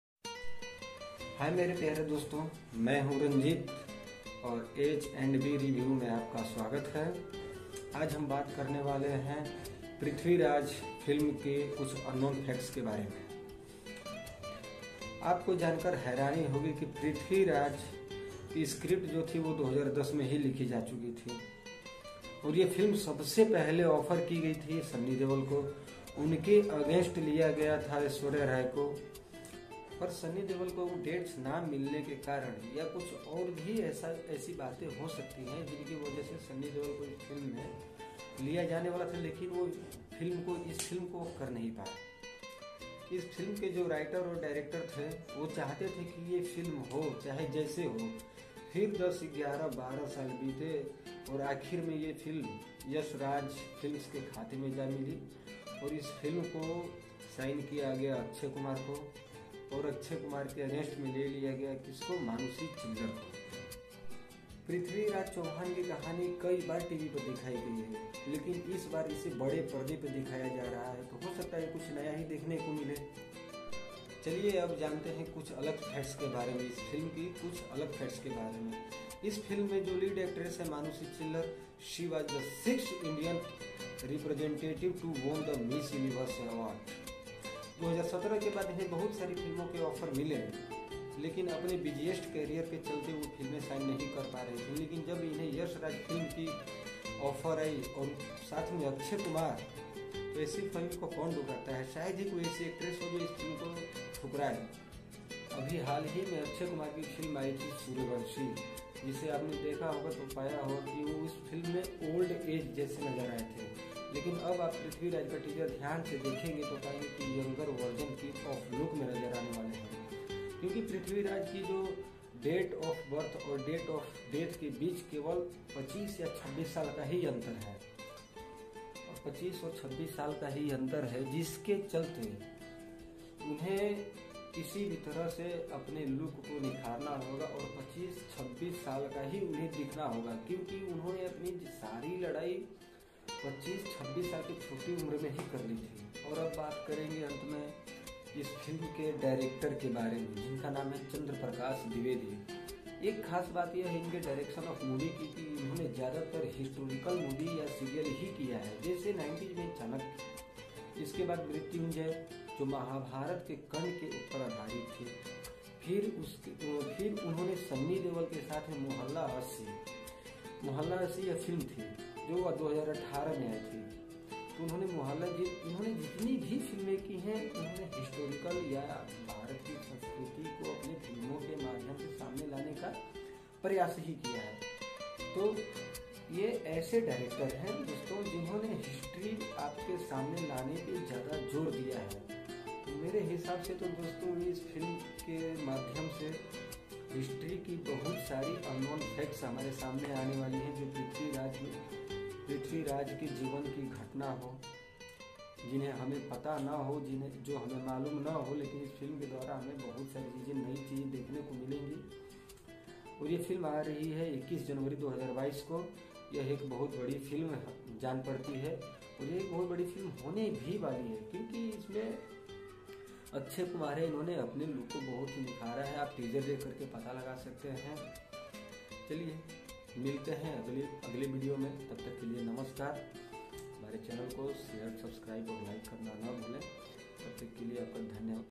PRITHVIRAJ Teaser Movie facts by Ranjeet. Akshay Kumar | Manushi Chilar | Sonu Sood
In this video all words are my own thoughts any type of entertainment will hurt you I am sorry for about

in this movie Akshay Kumar is in the role of Prithviraj